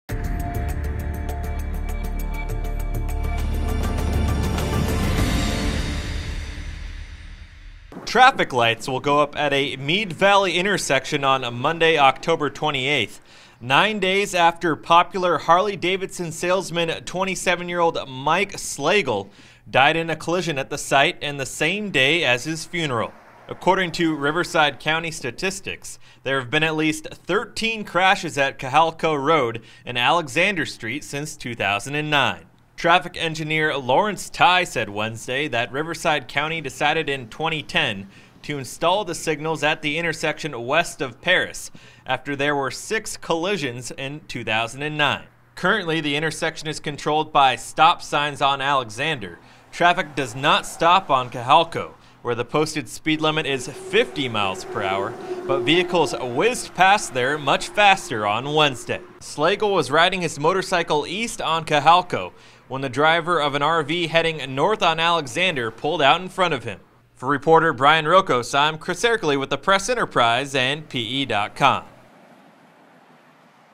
RIVERSIDE: Traffic signals going in at site of fatal crash
Traffic lights will go up at a Mead Valley intersection on Monday, Oct. 28 -- nine days after popular Harley-Davidson salesman Mike Slagle, 27, died in a collision at the site and the same day as his funeral. There have been at least 13 crashes at Cajalco Road and Alexander Street since 2009, according to Riverside County statistics.